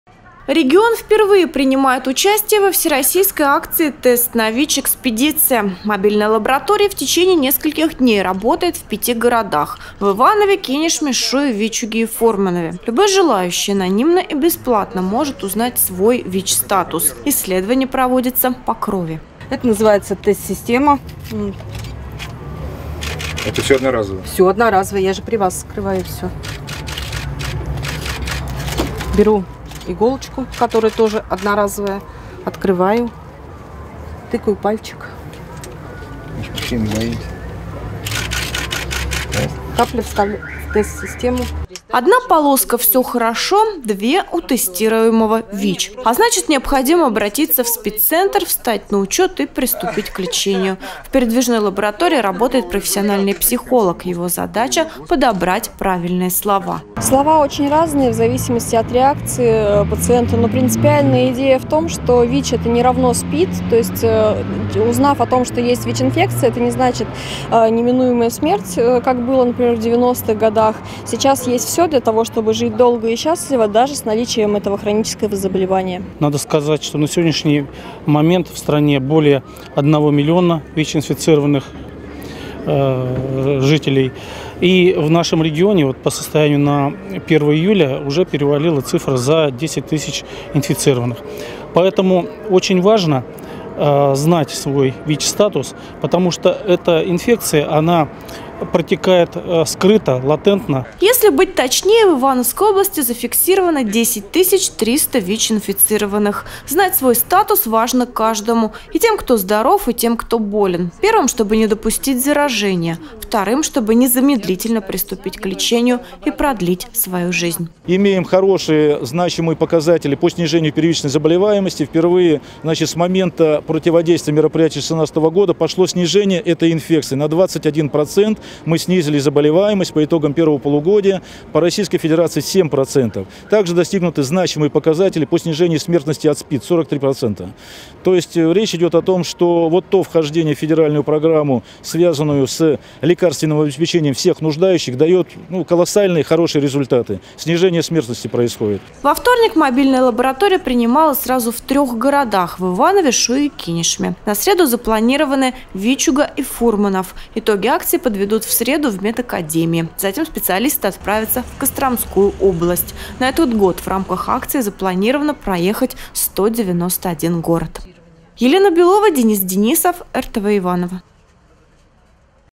Тест на ВИЧ: Экспедиция
Регион впервые принимает участие во Всероссийской акции «Тест на ВИЧ: Экспедиция». Мобильная лаборатория работает в Иванове, Кинешме, Шуе, Вичуге и Фурманове. Любой желающий анонимно и бесплатно может узнать свой ВИЧ-статус.